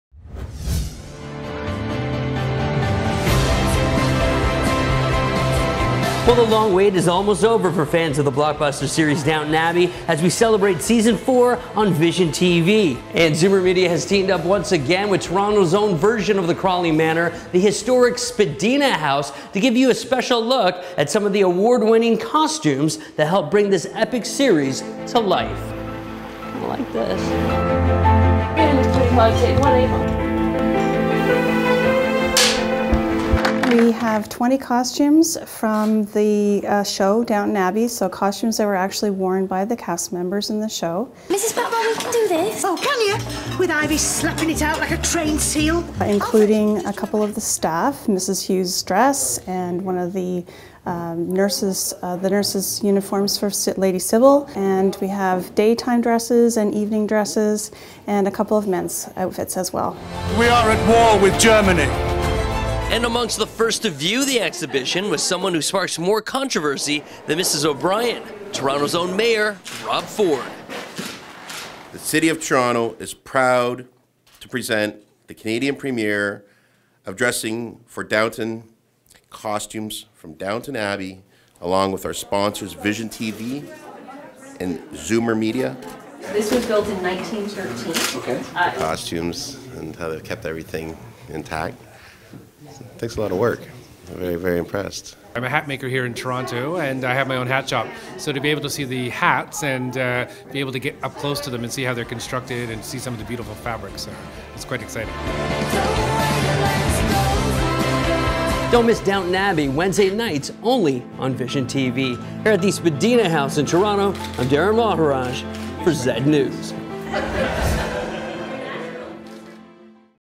ZNews - Dressing For Downton Abbey with Mayor Rob Ford
Some of the brilliant and beautiful costume designs previously worn by this blockbuster series' cast are on display for the Canadian premiere of the Dressing For Downton Exhibit at Spadina Museum, Toronto's Downton Abbey, from March 11 to April 13, Znews Videographer Darrin Maharaj has more...